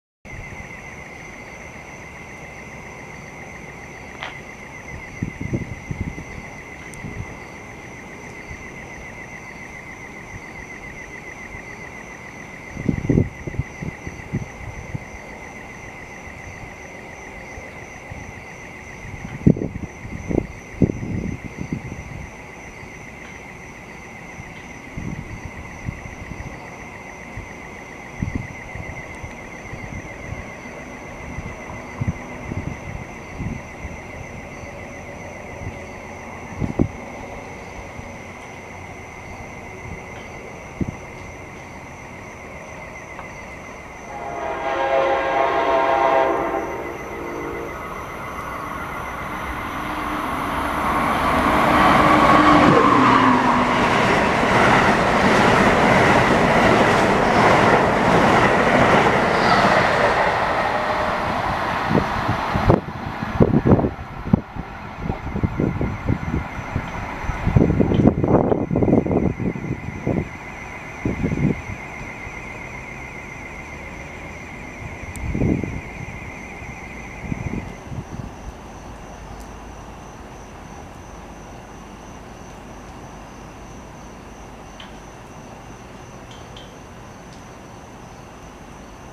A Late Amtrak Northeast Regional #138 at Hamilton, NJ
Shown racing past the rail station at Hamilton, NJ on the local track is Amtrak Northeast Regional #138 to New York running 34 minutes late with ACS-64 661 leading before I called it a night. The train is doing at least 110 mph & 138 has been delayed quite often due to speed restrictions south of Alexandria, VA.